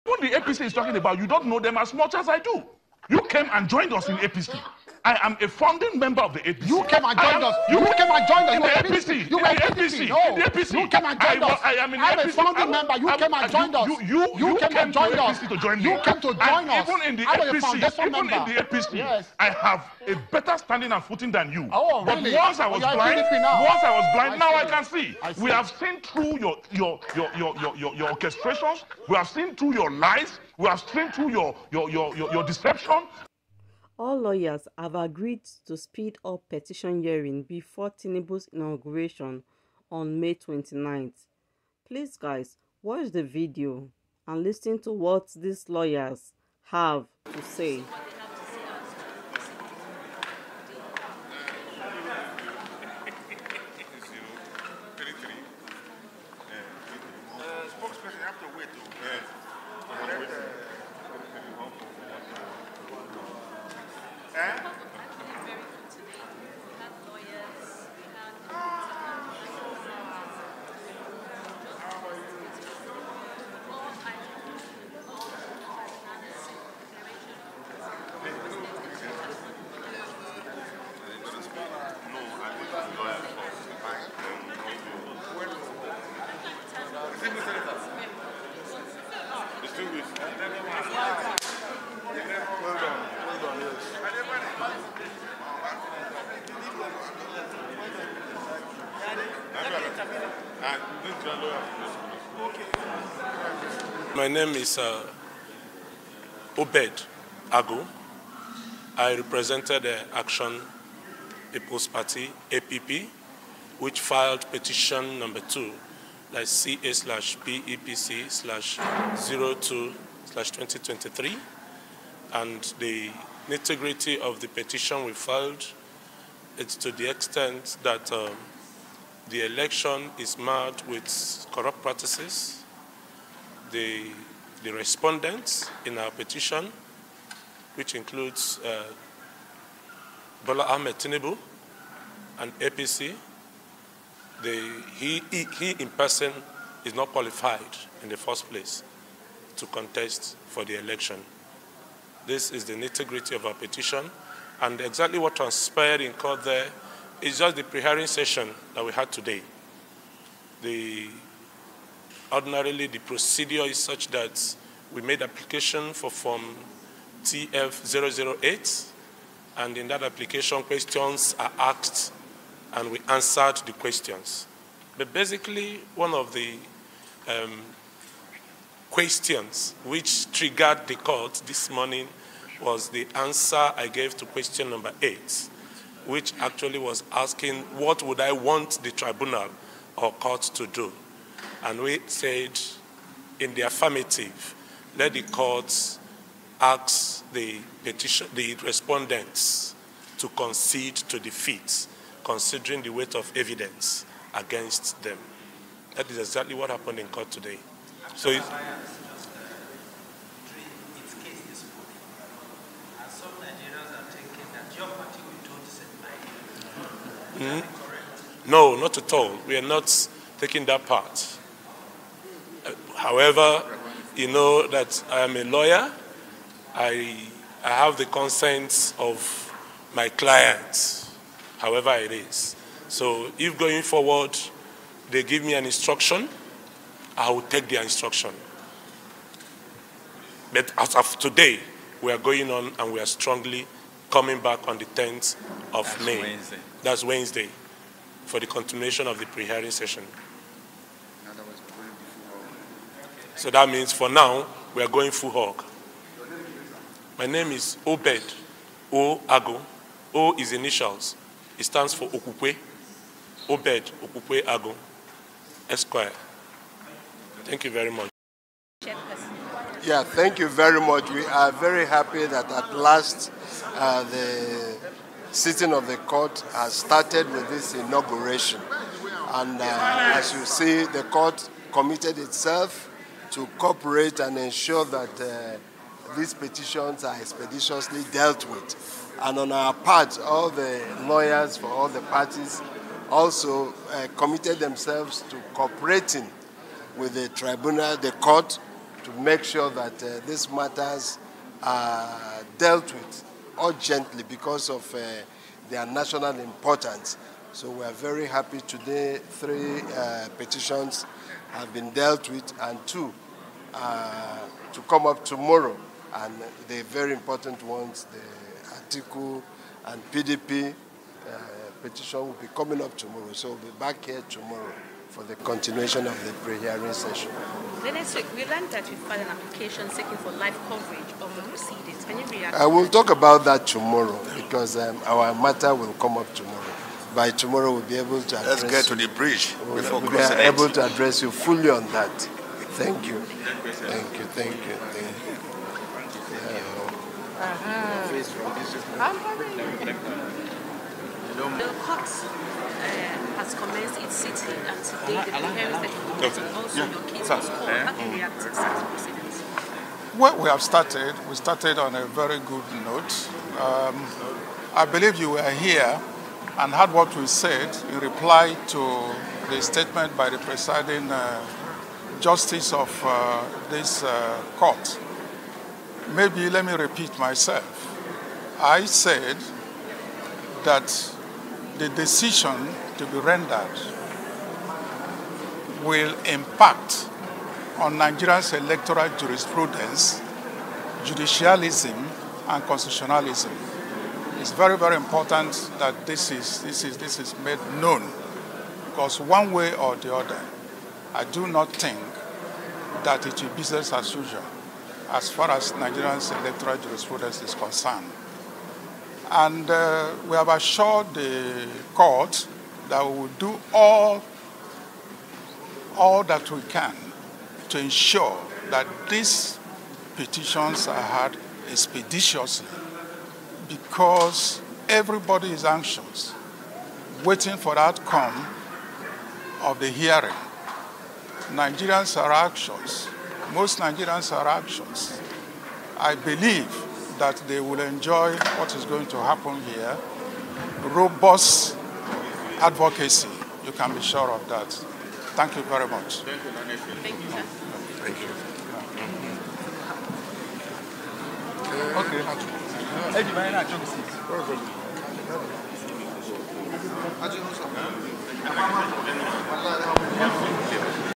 All Lawyers Agree To Speed Up Petition Hearing, To Stop Tinubu, Before May 29th!!tinubu
All Lawyers Agree To Speed Up Petition Hearing: Listen To Lawyers Working To Stop Tinubu, Before May 29th, Have To Say!!!